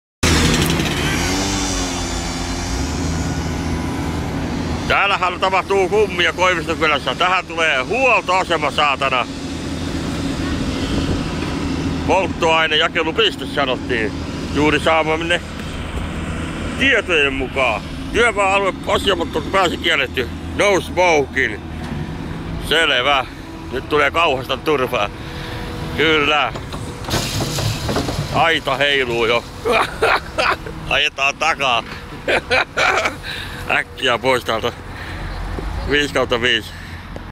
Hullu kaivin_kuski
niin..tulossa Kylmäasema tampereen prisman parkkipaikalle.eli polttoaine jakelupiste..